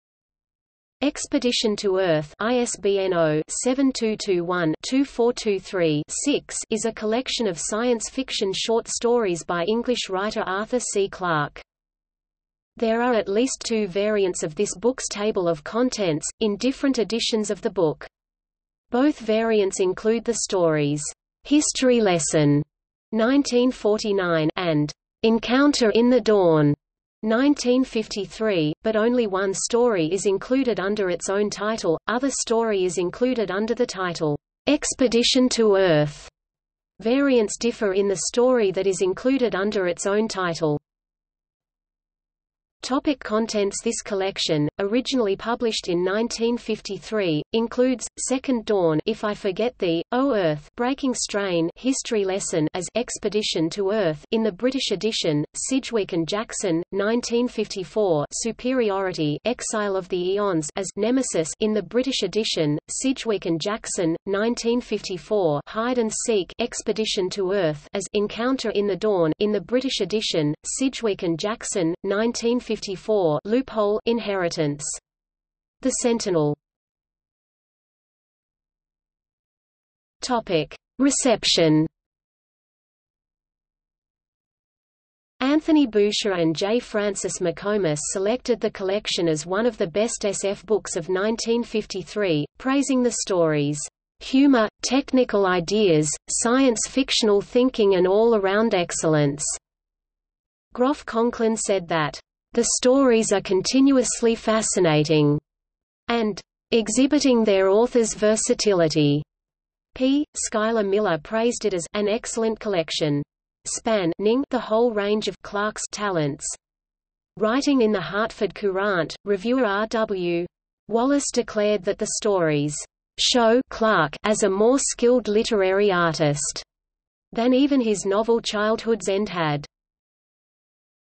Expedition to Earth | Wikipedia audio article
00:00:42 1 Contents
00:01:16 2 Reception

- Socrates

SUMMARY
Expedition to Earth (ISBN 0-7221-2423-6) is a collection of science fiction short stories by English writer Arthur C. Clarke.
There are at least two variants of this book's table of contents, in different editions of the book. Both variants include the stories "History Lesson" (1949) and "Encounter in the Dawn" (1953), but only one story is included under its own title; other story is included under the title "Expedition to Earth". Variants differ in the story that is included under its own title.